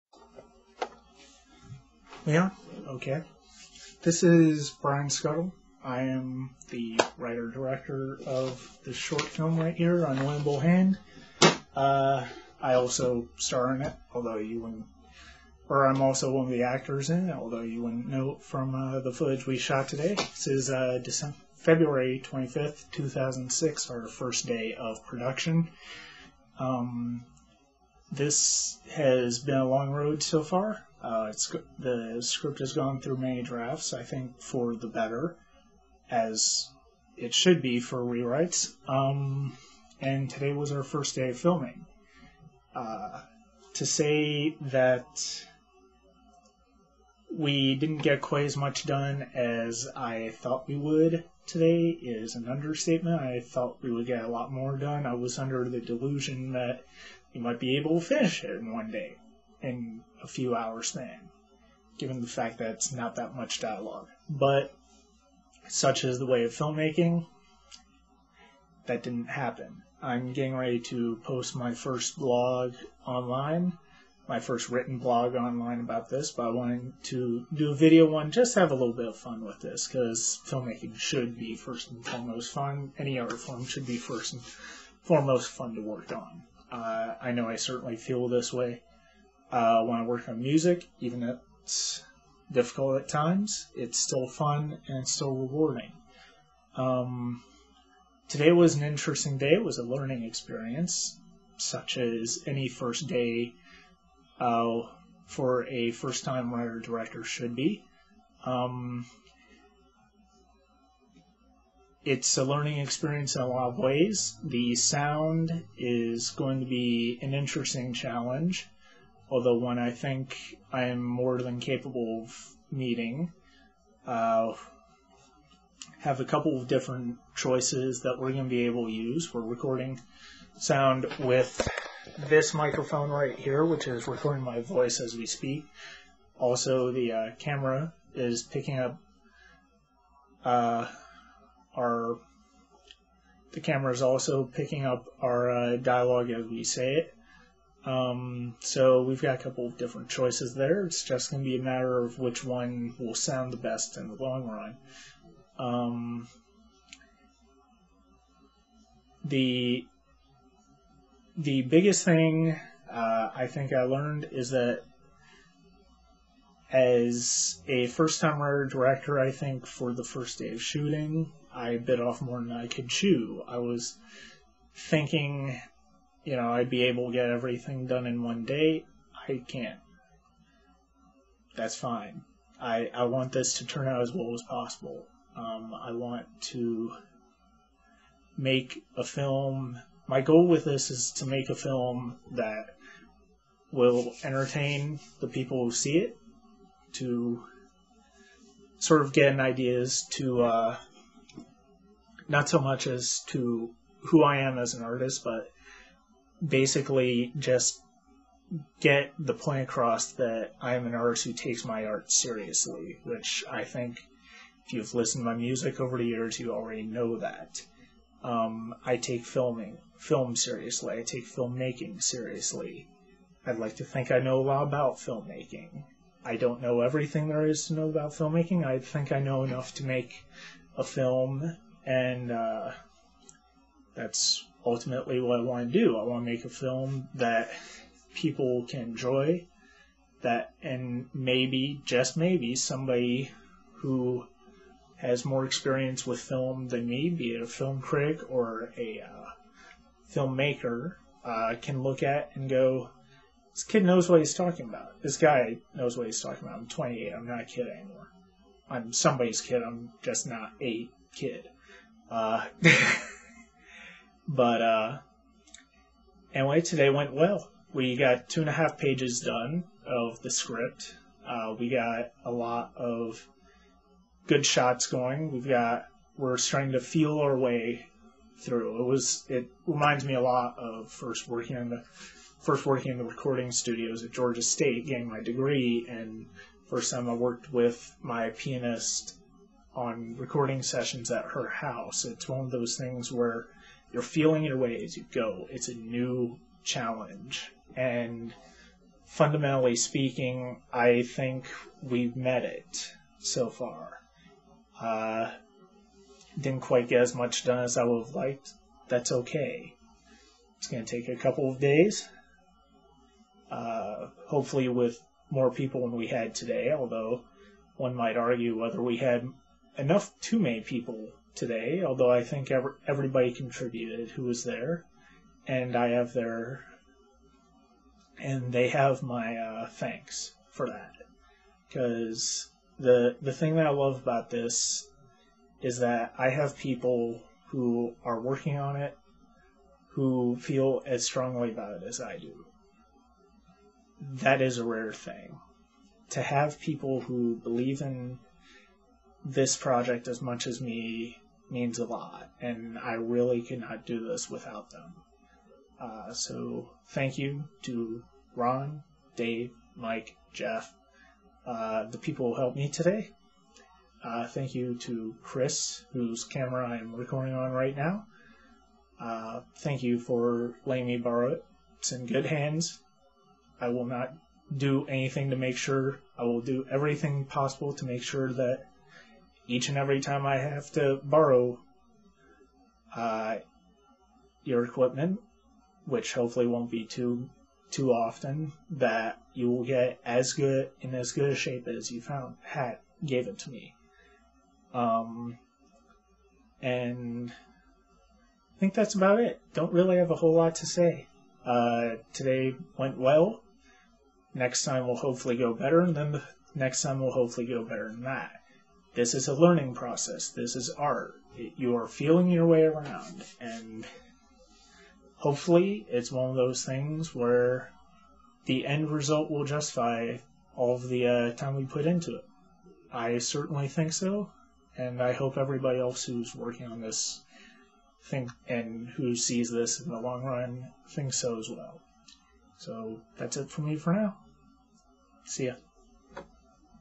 "Unwinnable Hand" Video Diary I
Filmed after our first day of filming, Brian pays homage to online production diaries in the craziest way...